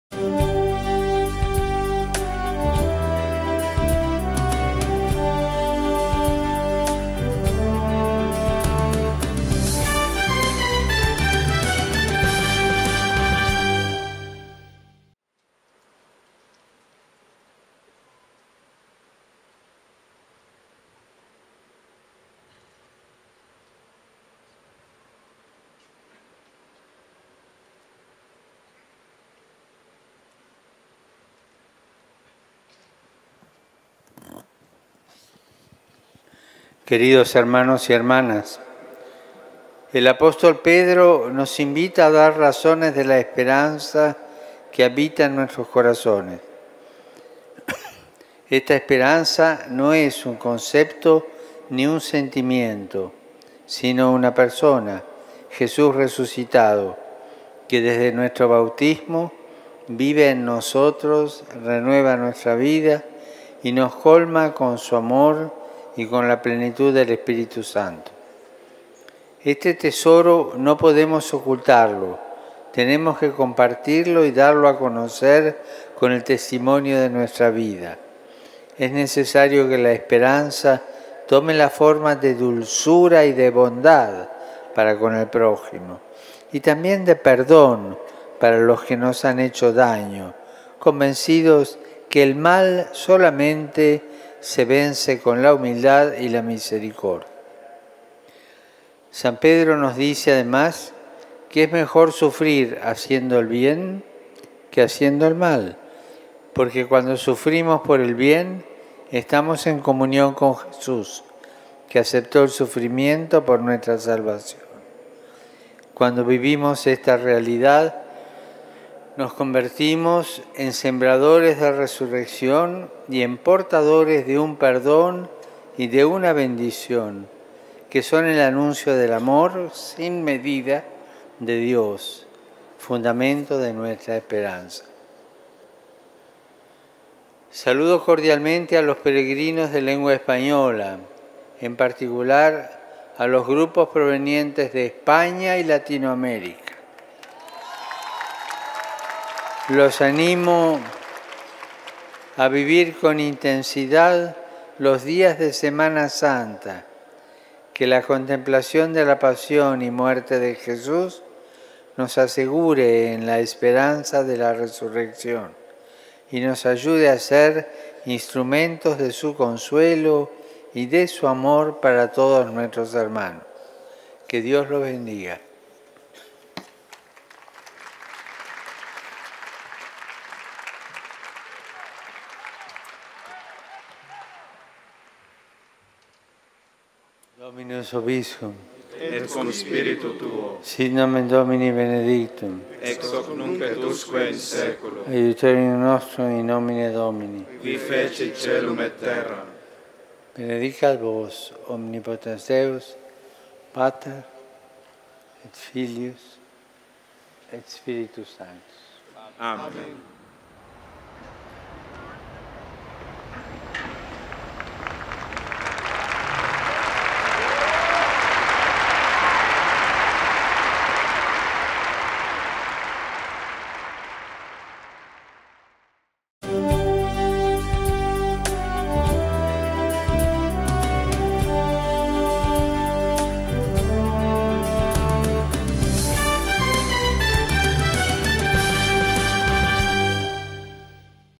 Catequesis en español del Papa Francisco 05/04/2017 HD
“El mal se vence sólo con la humildad, la misericordia y la mansedumbre”; lo afirmó el Papa en la catequesis del miércoles 5 de abril, en la quinta semana de Cuaresma, al reflexionar sobre la esperanza cristiana a partir, esta vez, de la lectura de la primera carta de San Pedro, quien invita a dar razón de nuestra esperanza. “Una esperanza que no es un concepto ni un sentimiento, sino una persona, Jesús resucitado, que vive en nosotros desde nuestro bautismo”.